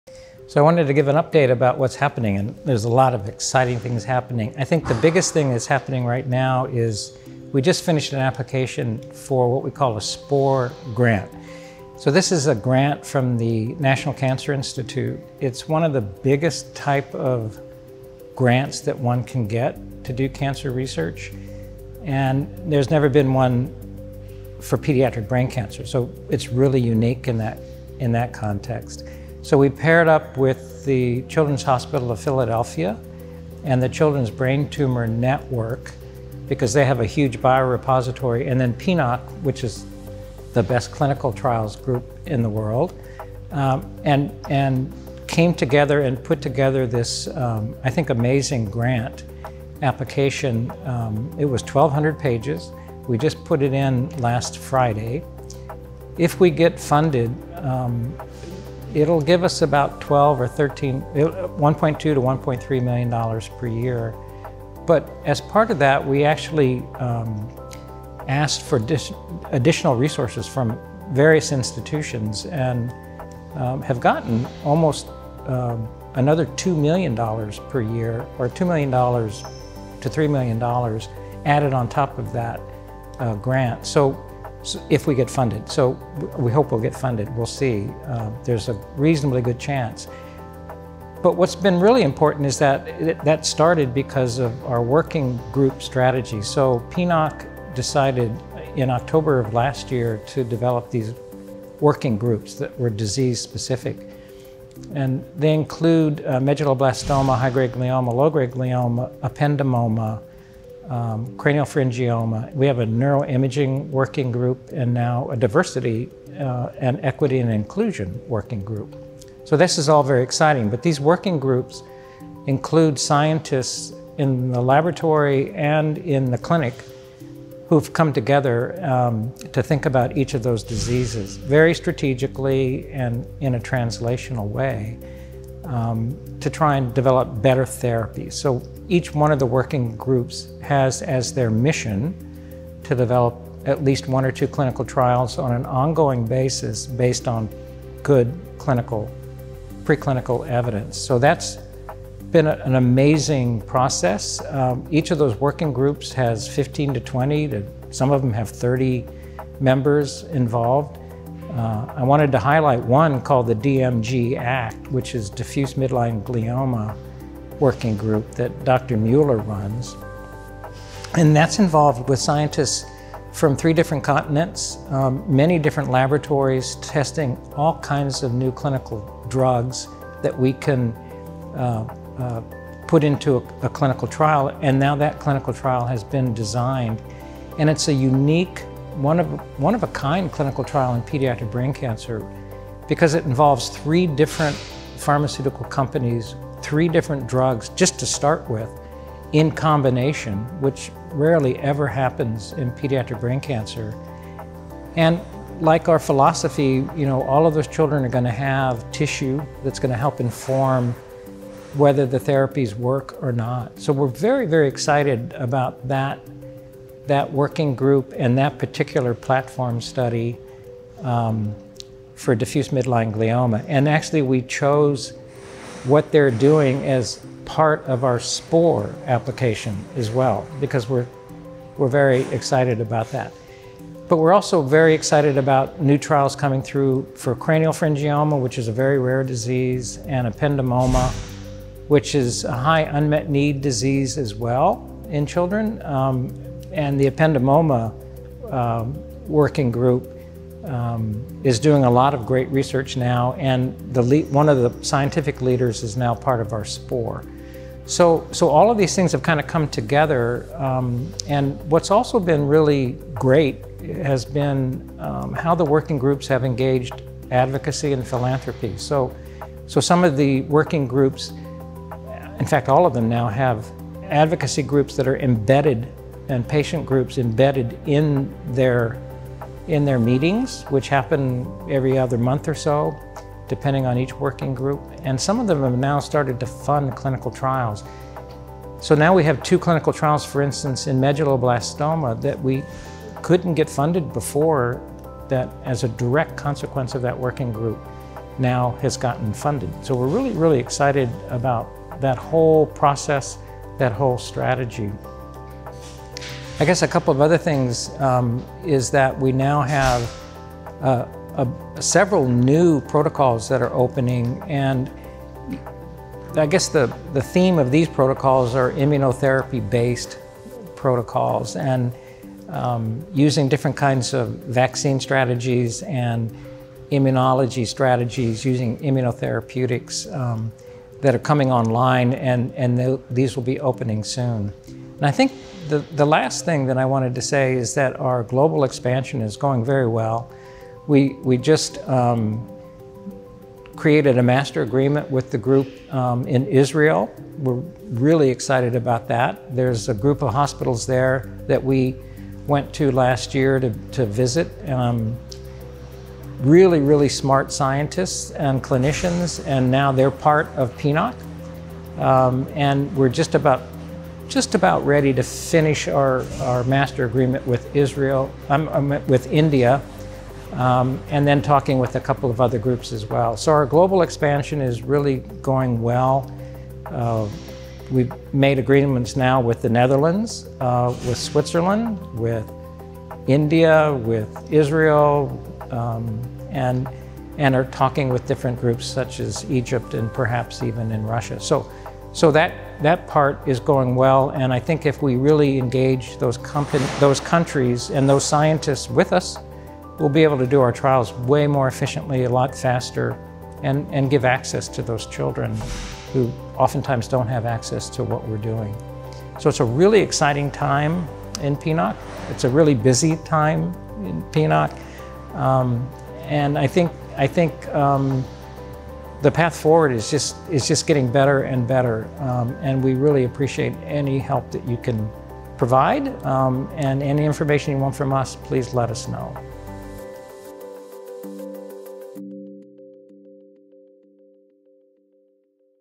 PNOC October 2020 Scientific Update With Dr. Michael Prados
Dr. Michael Prados is the visionary founder of The Pacific Pediatric Neuro-Oncology Consortium (PNOC) - an international consortium with study sites within the United States, Canada, Europe and Australia. PNOC are dedicated to bringing new therapies to children and young adults with brain tumors. PNOC's goal is to improve outcomes by translating the latest findings in brain tumor biology into better treatments for these children.

Support PNOC's critically needed pediatric brain cancer research and clinical trials by donating

For more information on PNOC's pediatric brain cancer research and clinical trials

Video credit: Matthew Prendergast